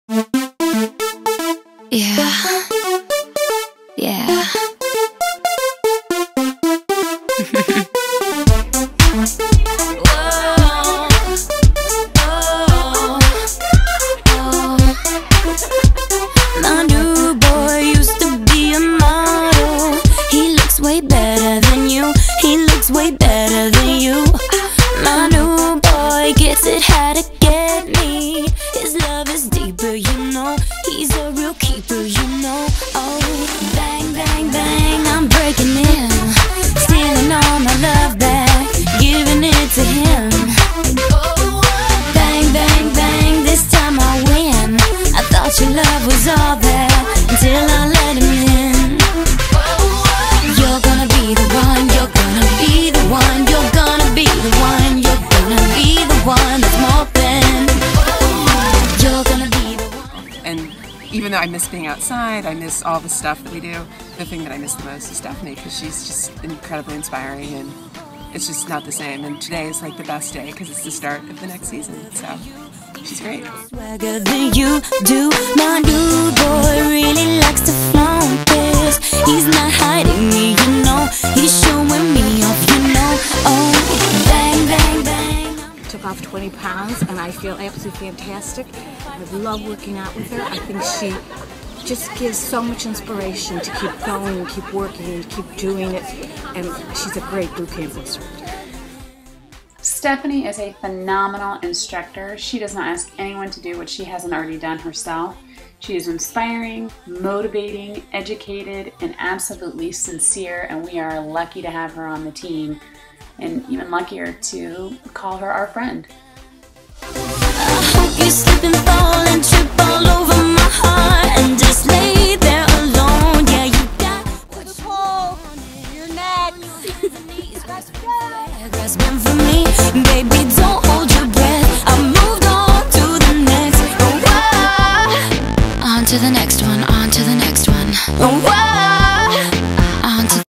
Stephanie
Stephanie Miller is a certified personal trainer through AFAA. She graduated from the University of Michigan and went on to get her JD from The John Marshall Law School in Chicago, Illinois. After taking time off from the practice of law to raise her three boys, she has embarked on her second career and passion, fitness!
Over the past several years Stephanie has enjoyed exercise and acquired extensive knowledge that she loves to share with others in their quest to get (and stay) fit. Currently, Stephanie teaches bootcamps and does individual and group training. Stephanie's enthusiasm and drive makes working out with her a lot of fun as well as challenging!